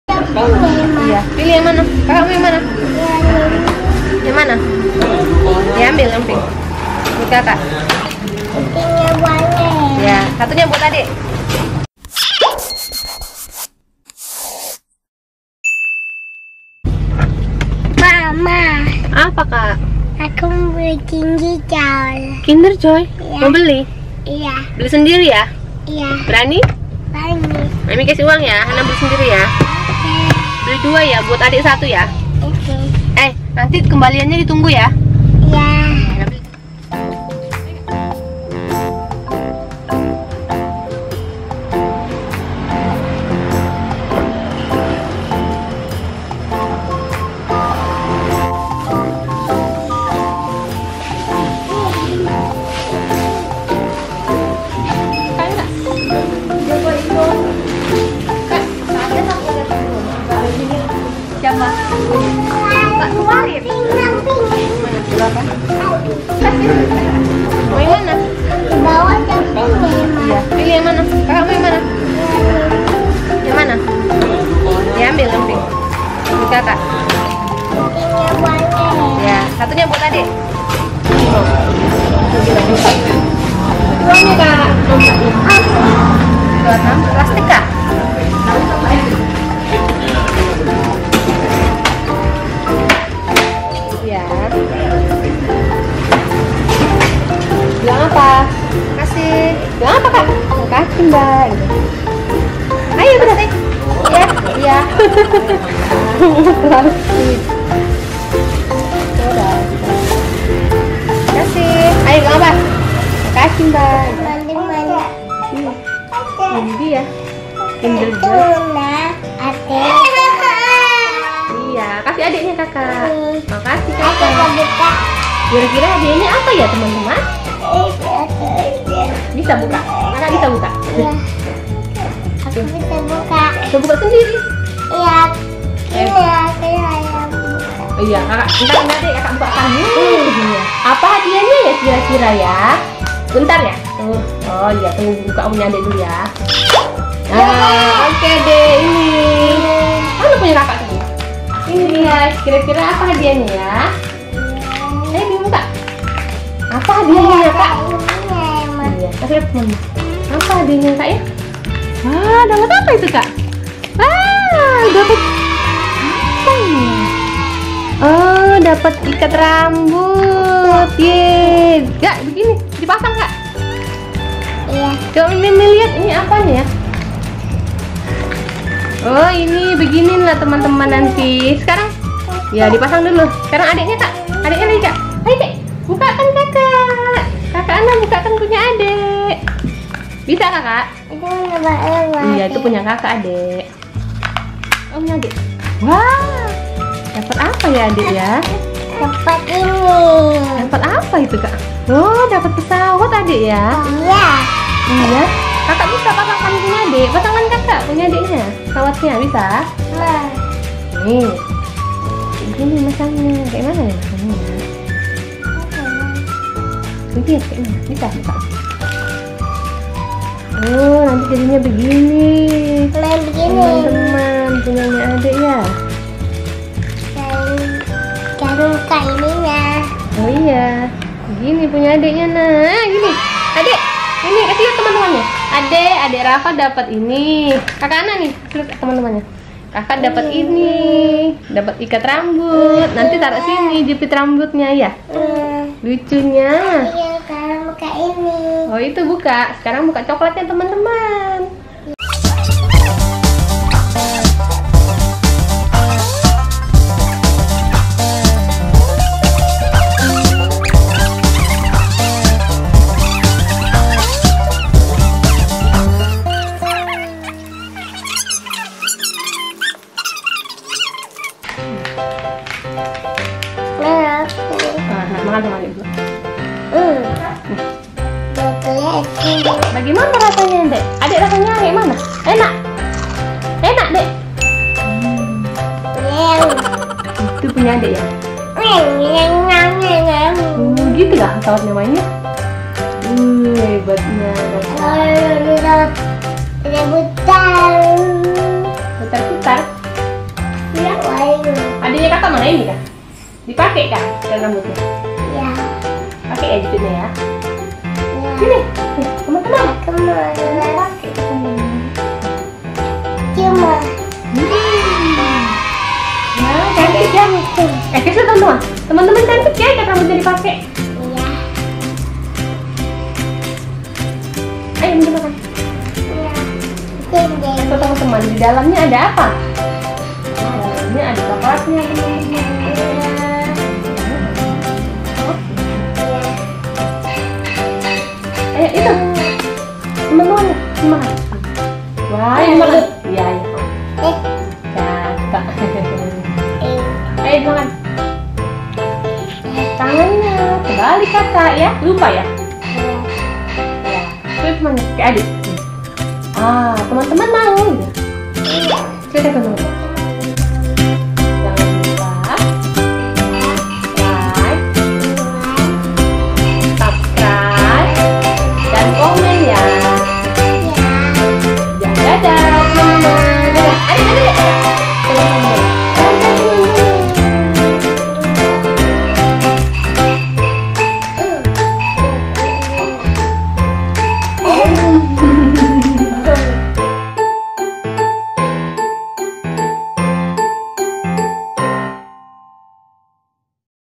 Hana Beli Kinder Joy 🐣 Apa Hadiahnya?🎊 Surprise Egg Mainan Anak
Habis cari sarapan bubur ayam dalam perjalanan pulang Hana minta beli Kinder Joy di minimarket dekat rumah. Karena masih di Kalimantan dan disini tidak ada Alfamart atau Indomaret hehehe.

Akhirnya mama berinisiatif agar Hana belajar percaya diri dan membeli Kinder Joy sendiri. Meski demikian mama tidak tega dan tetap mendampingi Hana namun pembelian dan pembayaran dilakukan oleh Hana sendiri.

Kinder Joy sendiri tentunya sudah tidak asing bagi-teman-teman. Jajanan anak yang berisi coklat namun disertai hadiah seperti surprise egg.  Isi hadiahnya yang berupa mainan anak yang masih menjadi misteri sampai Kinderjoy dibuka.

Setelah beli Kinder Joy sendiri akhrinya Hana langsung pulang kerumah dan memutuskan untuk membuka isi Kinder Joy di rumah saja. Kira-kira apa isinya? ayo kita lihat sama-sama ya teman-teman.